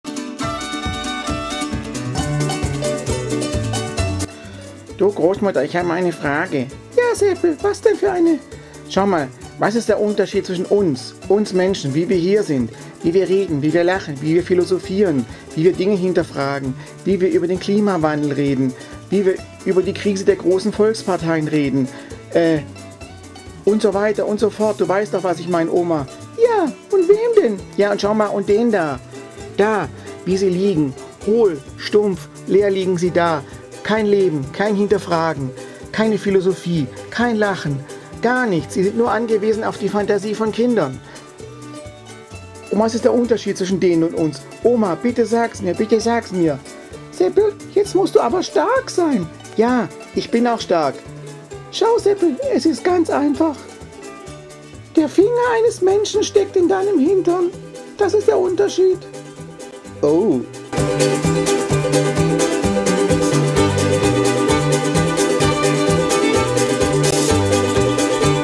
Wie man mit dem Finger philosophiert - oder Fingerpuppen und Metapyhsik
Oma und Bernd unsere wackeren Helden wagen die große Abrechnung mit den führenden Weltreligionen. Außerdem erklären Sie in knapp einer Minute den Sinn des Lebens